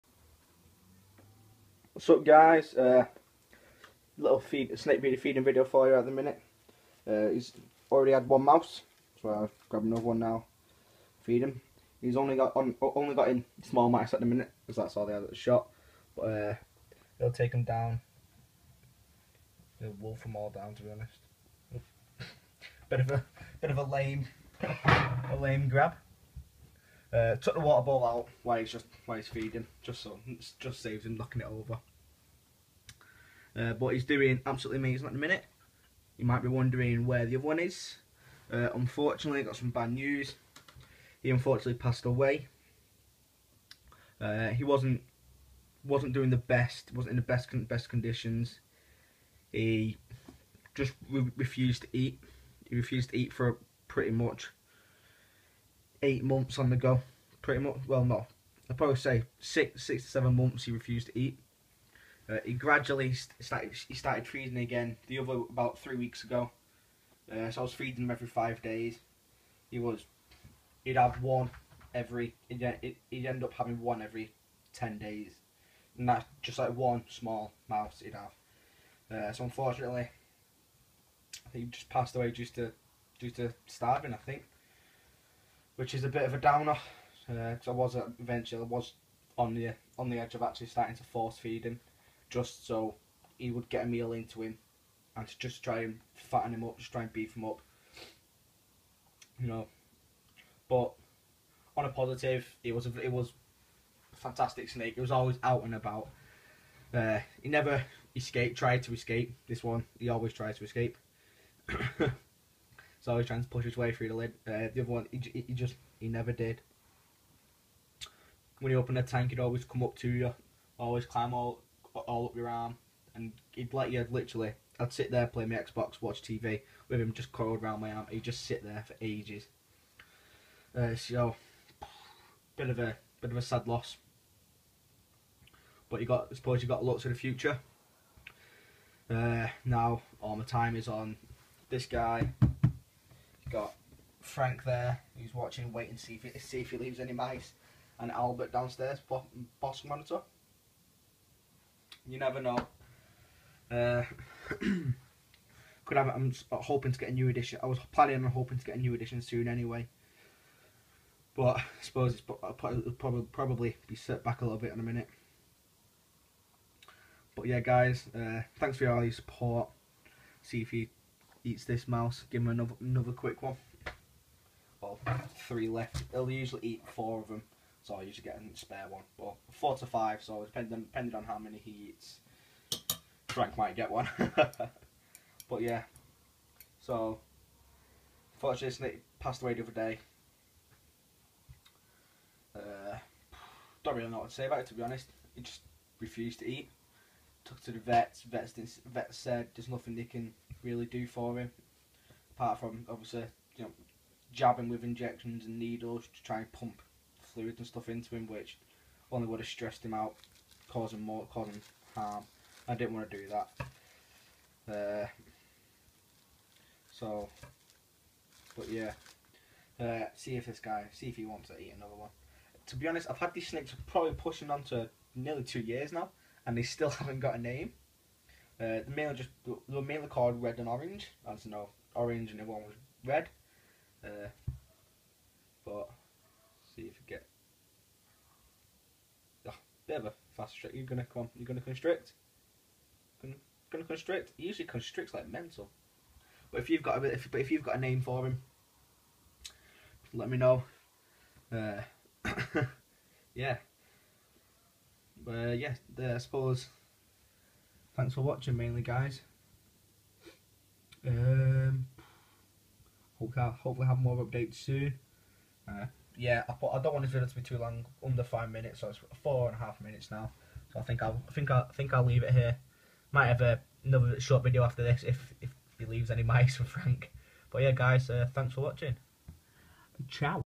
Snake feeding video
A quick video of my snake eating some small mice; unfortunately one of my snake has passed away as he refused to eat for so long. However you have to look towards the future and not dwell on the past.